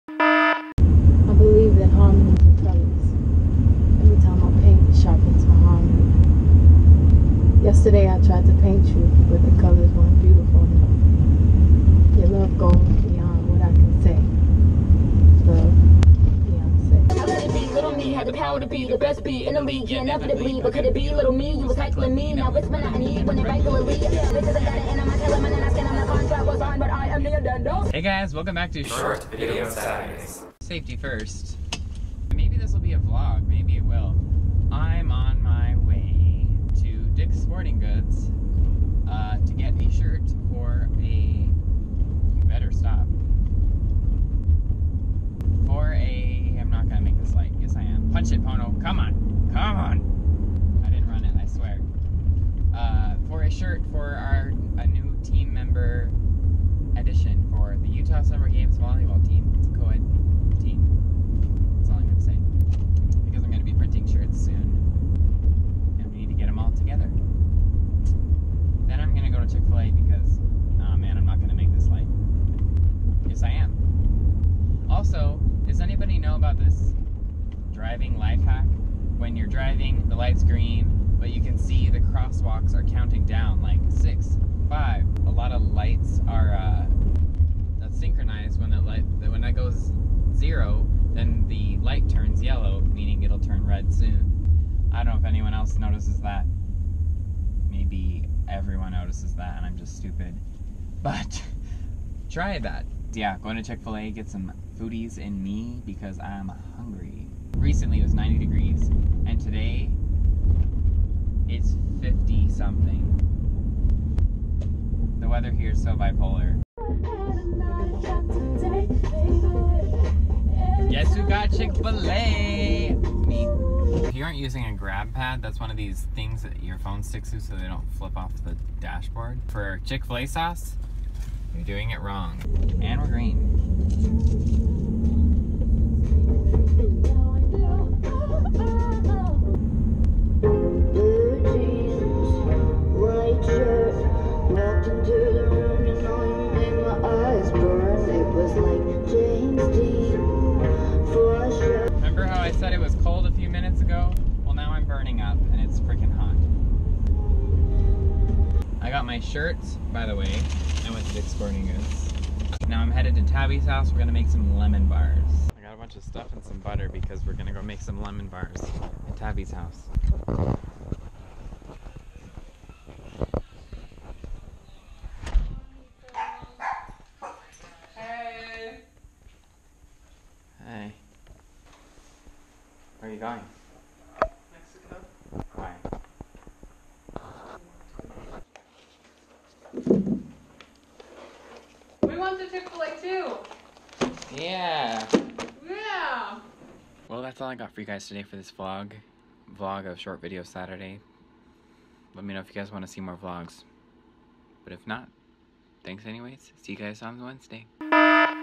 Car Karaoke!!!
Going to Dicks sporting goods & Chick fil a.